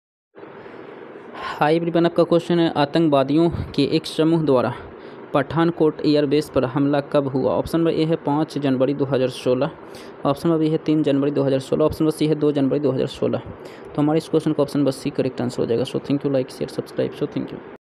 आतंकवादियों के एक समूह द्वारा पठानकोट एयरबेस पर हमला कब हुआ?
(a) 5 जनवरी, 2016

(b) 3 जनवरी, 2016

(d) 6 जनवरी, 2016